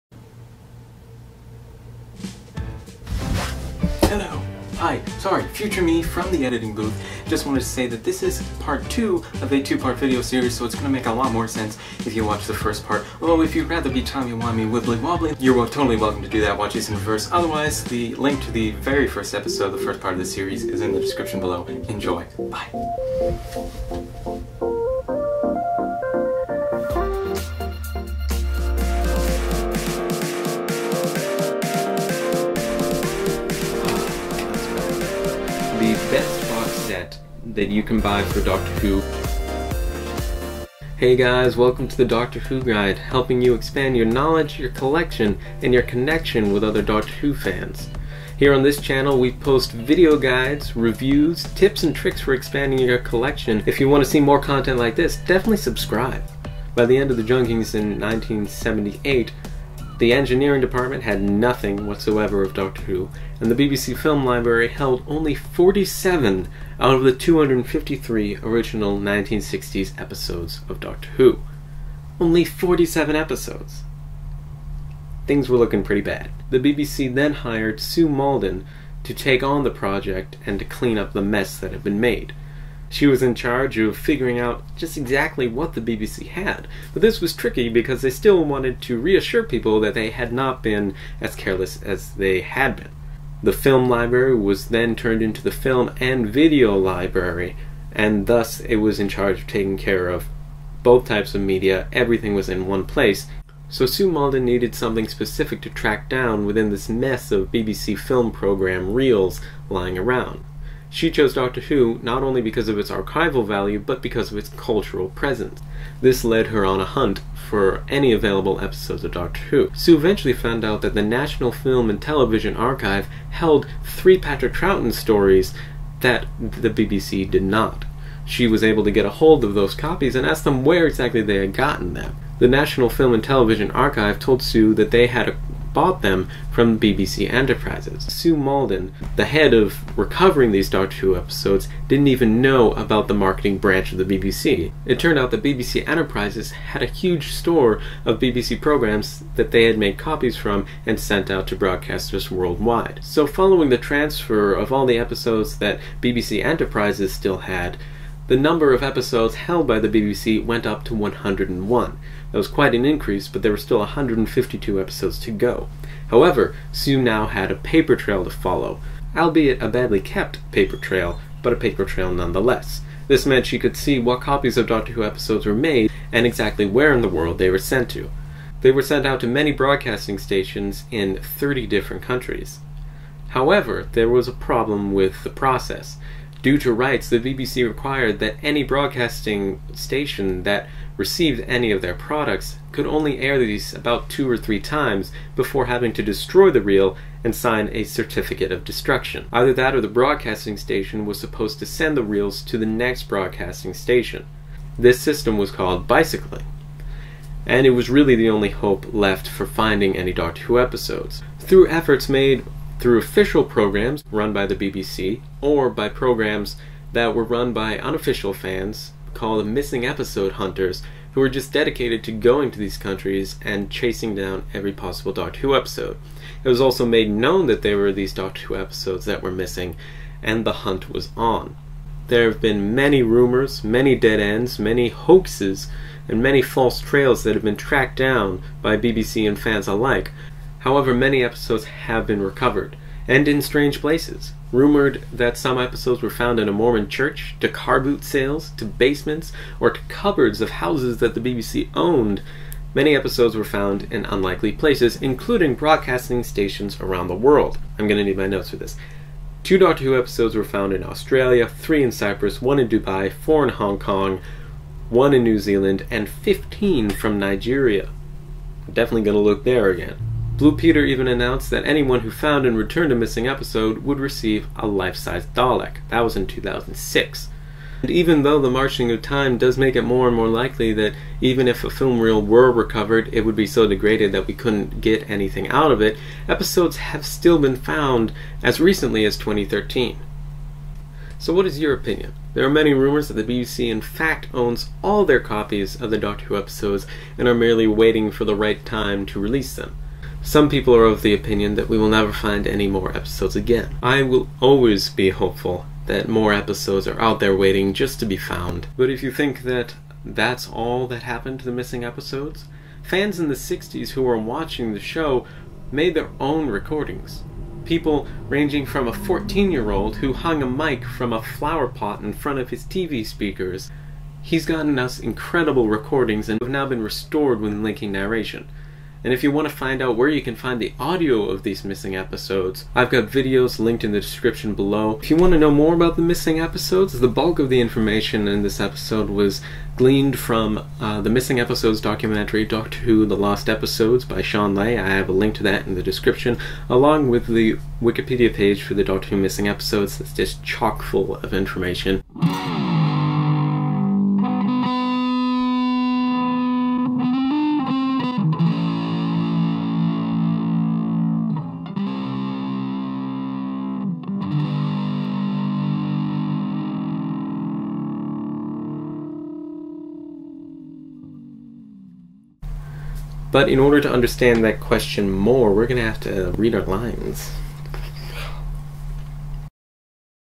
Doctor Who Missing Episodes - How Did They Find Them? - Part 2
Alex examines how the doctor who missing episodes were found!

Resources I found invaluable when researching the doctor who missing episodes were:
Wikipedia Page for the Missing Episodes.

QUESTION — Did I get anything wrong? Were there any details that I missed? I want to know! Also what missing episodes do you want found the most?

About this video:
In this video Alex Patterson from The Doctor Who Guide talks about how the doctor who missing episodes went were found. This is the second part of his two part video series on how the dr who missing episodes went missing.